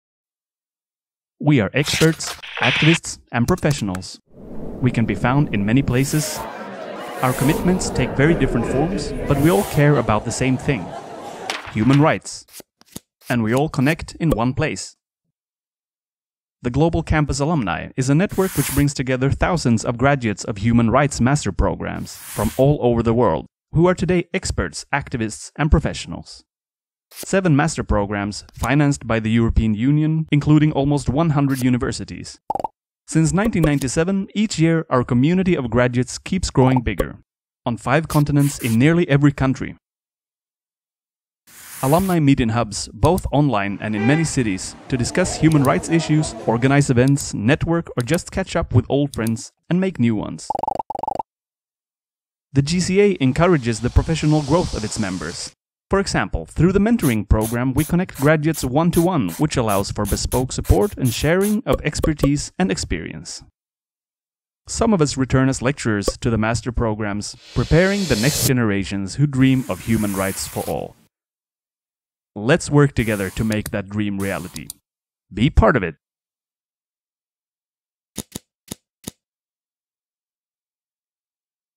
Global Campus Alumni
The Global Campus Alumni (GCA) is an international organisation (AISBL) registered in Brussels, Belgium. It is a network of six alumni association of human rights master programmes gathering almost 2,000 experts, activists and professionals spread throughout the world. The master programmes the alumni that graduated from them are all part of the Global Campus of Regional Human Rights Master Programmes funded by the European Commission and led by the European Inter-University Centre for Human Rights and Democratisation (EIUC), based in Venice Italy.